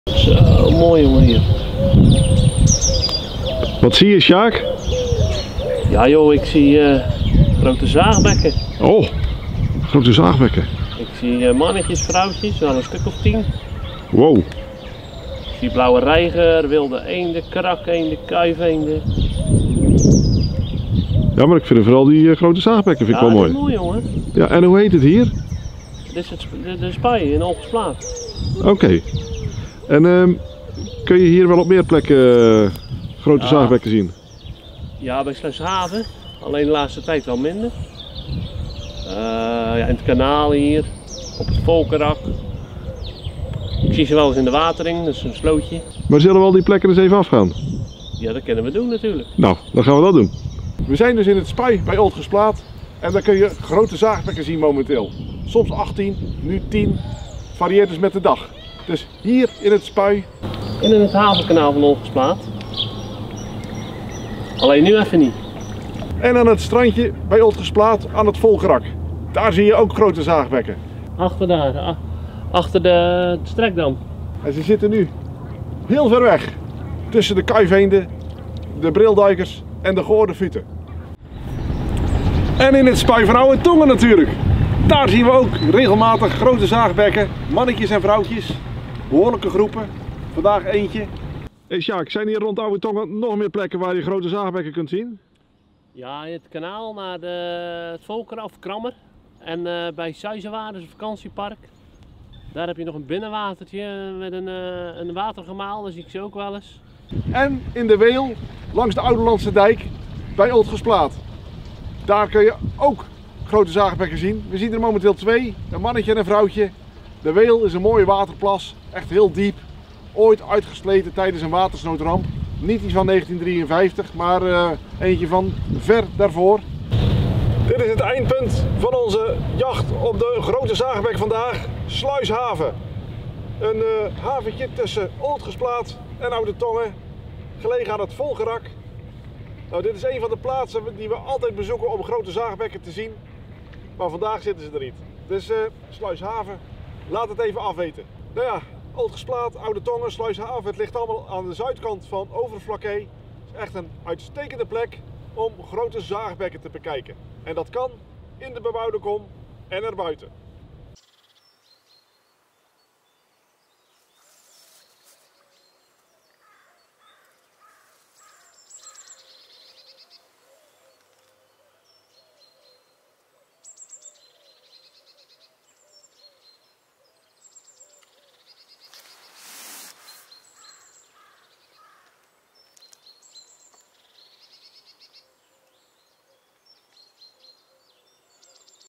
Vlog N° 244 Grote zaagbekken op Oostflakkee / Great Merganser
Lees meer over de locaties die we in deze vlog bezochten

Vandaag gingen Sjaak en ik op zoek naar grote zaagbekken op Goeree-Overflakkee, en preciezer: op Oostflakkee. Want we beperkten ons tot de dorpen Ooltgensplaat en Oude-Tonge, twee prima plekken om grote zaagbekken te zien. Hier overwinteren elk jaar behoorlijke aantallen grote zaagbekken die je soms van behoorlijk dichtbij kunt zien. Neem bijvoorbeeld in beide dorpen 'het spui'. Redelijk diepe wateren, behoorlijk breed en nog binnen de bebouwde kom ook. Ideaal terrein voor grote zaagbekken. En omdat daar regelmatig mensen voorbij lopen of fietsen zijn de schuwe zaagbekken redelijk gewend aan mensen. Maar loop niet zomaar naar het water, want dan vliegen ze geheid op. Grote zaagbekken blijf ik bijzonder vinden. Het heeft lang geduurd voordat ik mijn eerste grote zaagbek zag en dat was natuurlijk in Ooltgenplaat waar we zeven jaar hebben gewoond.

Today Sjaak and I went looking for great mergansers on Goeree-Overflakkee, and more precisely: on Oostflakkee. We limited ourselves to the villages of Ooltgensplaat and Oude-Tonge, two great places to observe great mergansers. Significant numbers of large mergansers spend the winter here every year, which you can sometimes see from quite close. Take 'het spui' in both villages, for example. Fairly deep waters, quite wide and within built-up areas as well. Ideal terrain forgreat mergansers. And because people regularly walk or cycle past there, the shy mergansers are fairly used to people. But don't just walk to the water, because then the birds will definitely fly up. I continue to find great mergansers special birds. It took a long time before I saw my first great merganser and that was of course in Ooltgenplaat, where we lived for seven years.